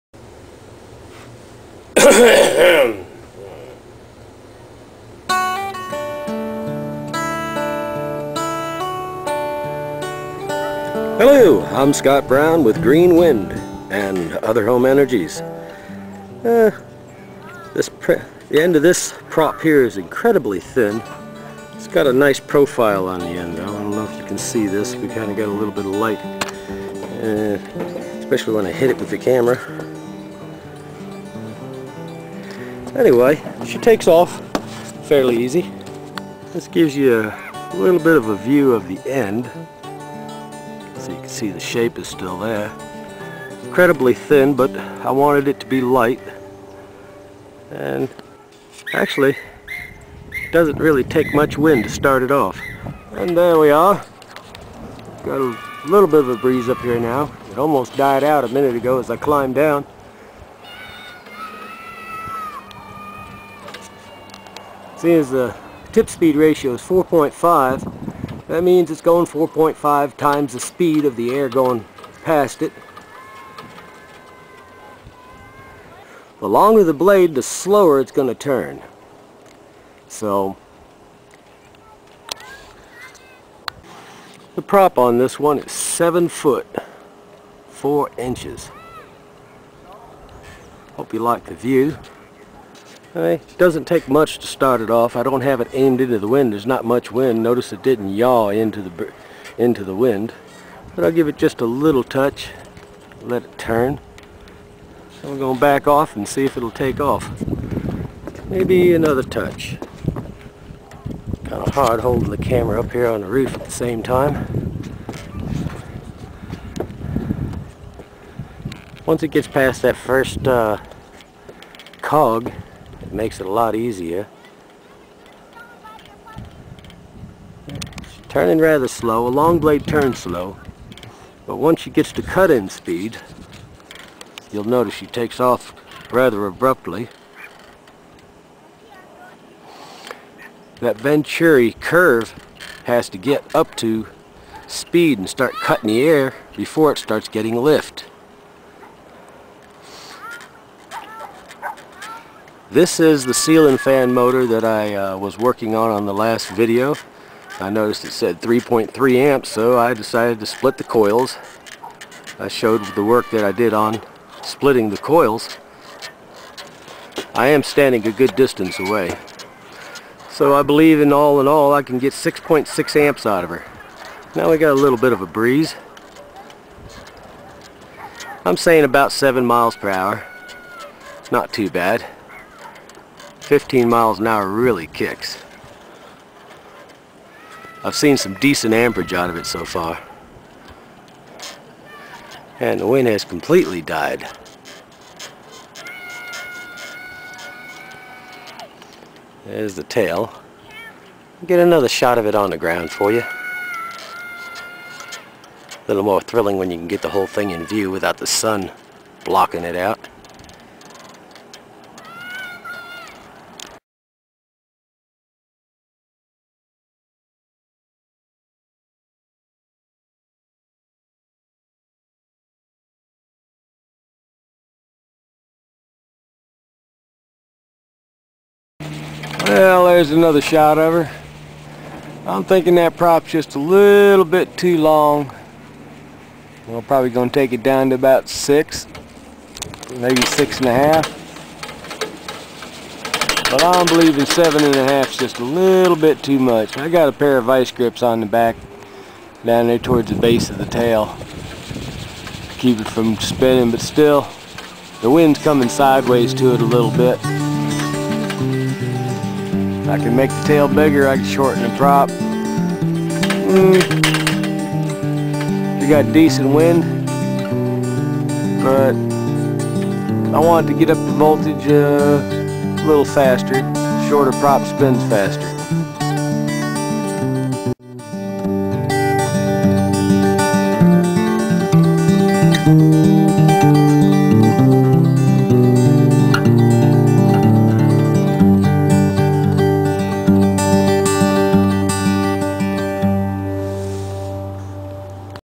ANALYSIS OF A WIND TURBINE FROM HARBOR BREEZE CEILING FAN MOTOR
JUST SOME SPARE FOOTAGE DECIPHERING THE CLUES I GET FROM THE PERFORMANCE OF THIS NEW ONE. AND BALANCING THE INFO WITH WHAT I HAVE EXPERIENCED BEFORE. I HAVE DONE SOME OF THESE IMPROVEMENTS AND I HAVE THE RESULTS IN A NEW VIDEO COMING SOON. ITS ALL GOOD NEWS.MUDDy